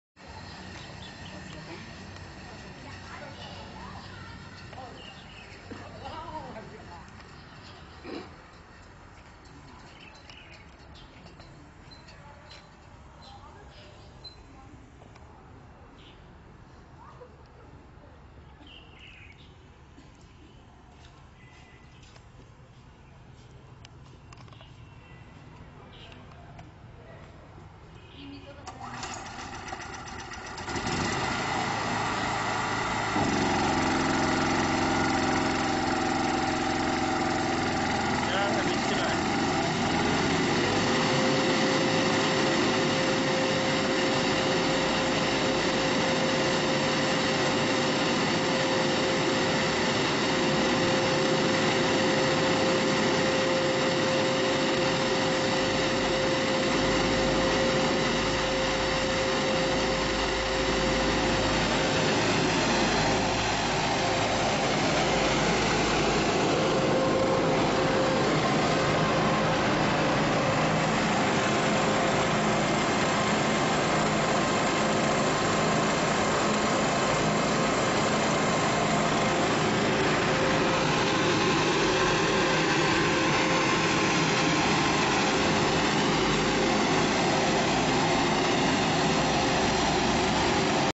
3500M 2.8kw generator with SUPER LOADING CAPACITY感性发电机 2
Our unique "SUPER LOADING CAPACITY gasoline generator".
For example, 2.8kw generator can ran 4kw of light bulbs or start and ran a 2kw air conditioner without influencing the life time of the generator itself.
DIGITAL AVR, Several USD difference in price comparing to traditional generators.
Skype: sbl4063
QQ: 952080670
Factory: No.2 Aokang Road, Bishan Industry Park, Chongqing, China.